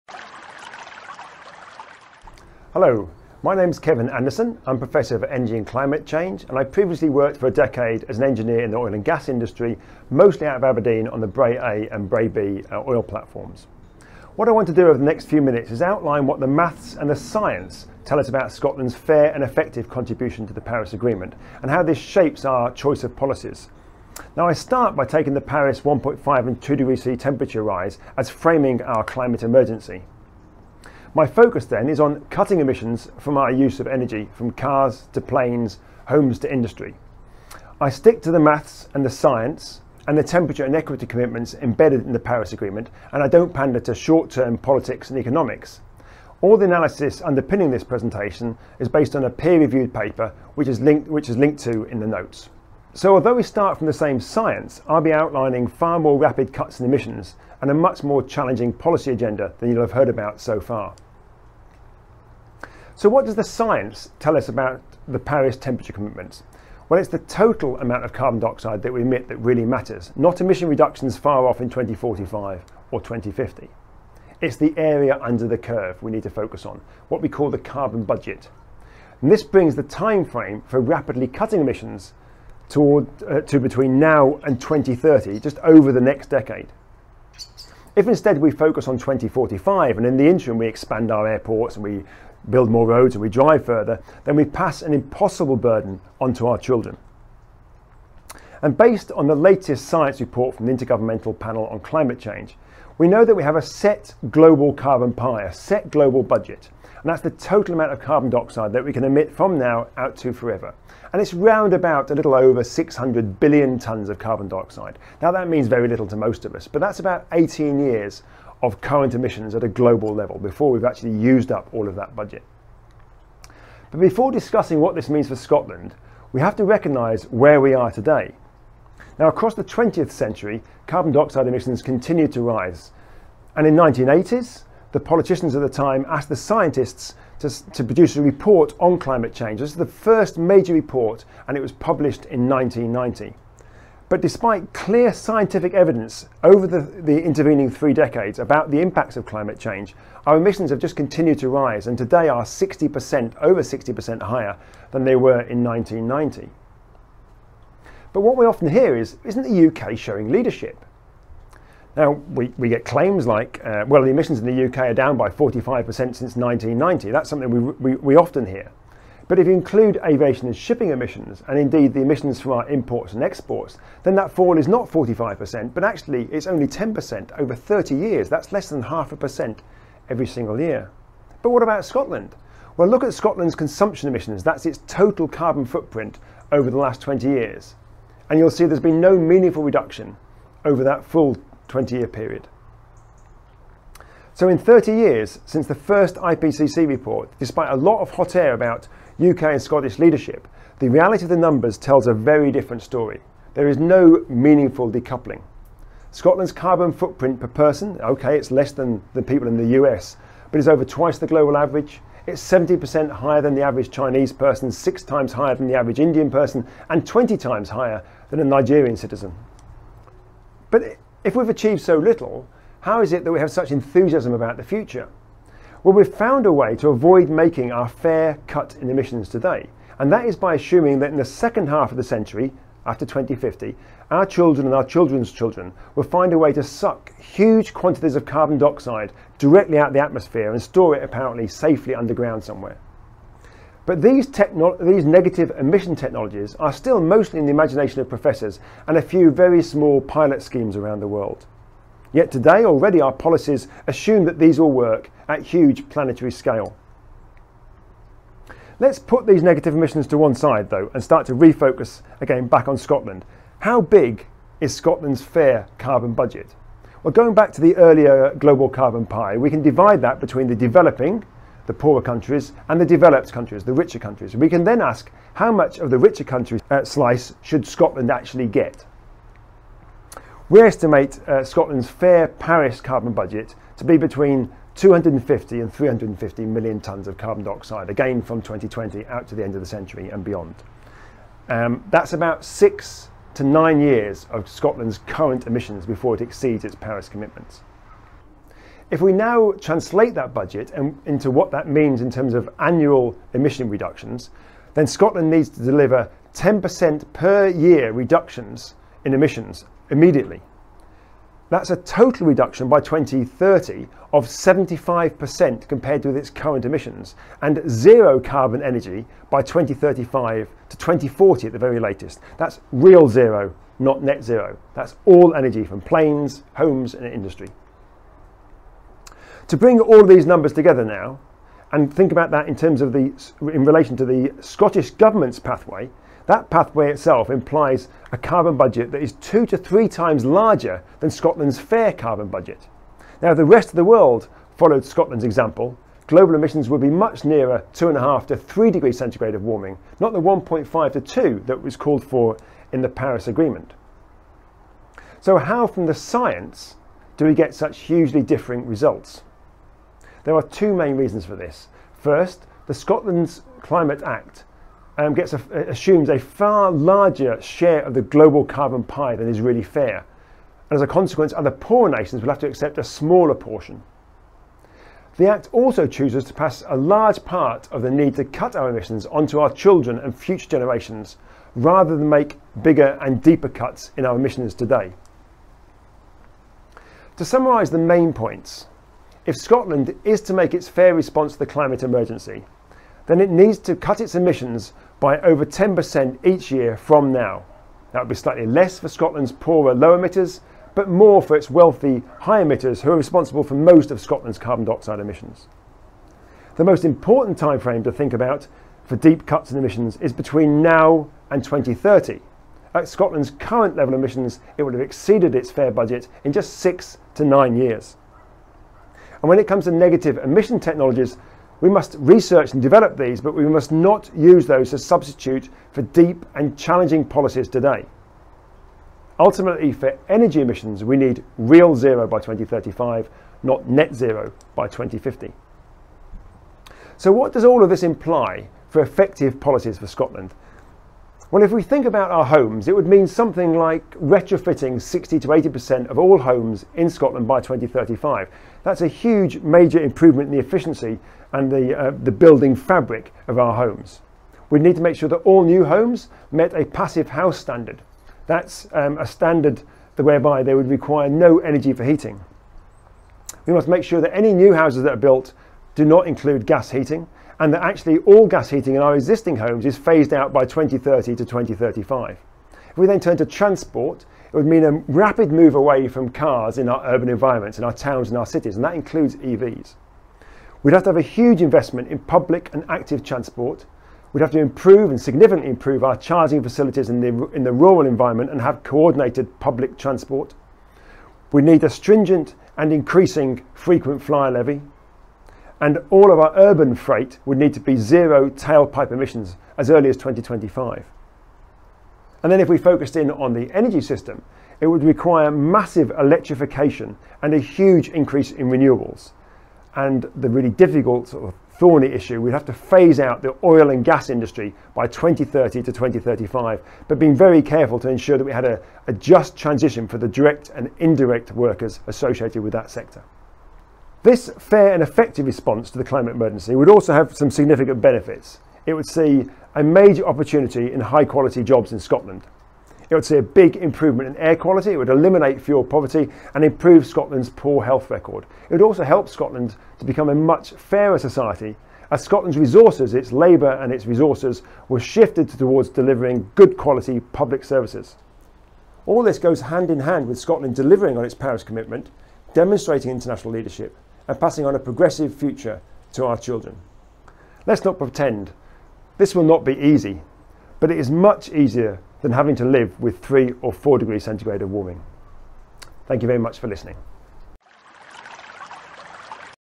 What does the Paris Agreement mean for Scotland? with Kevin Anderson | Scotland's Climate Assembly
In Weekend 2, Scotland's Climate Assembly heard from Prof Kevin Anderson about  Scotland's fair share of emissions.

The paper Kevin refers to is 'A factor of two: how the mitigation plans of ‘climate progressive’ nations fall far short of Paris-compliant pathways', Climate Policy 2020.

A more digestible version of the paper is available as a blog

Scotland’s Climate Assembly brings together people from all walks of life, from across Scotland, to learn about and discuss how we can tackle climate change.